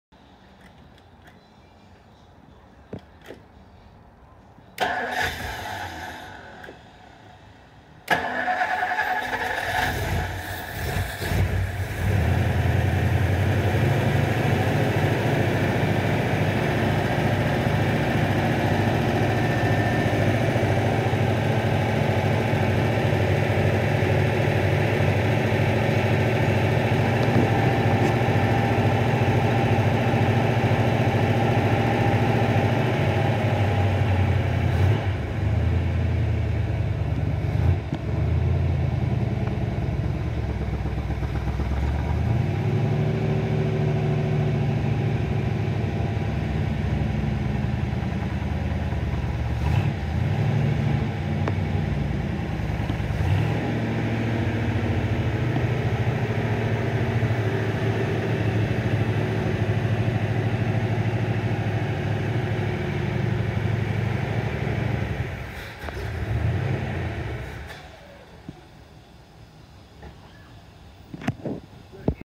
8EE1A776 96FA 4331 9700 820FE55E0214
Completely rebuilt and bored .30 over 312ci 57’ T-Bird engine fired off for the first time since 1986. Jim’s Classic Garage will be delivering the car to the client this week. Smiles all around.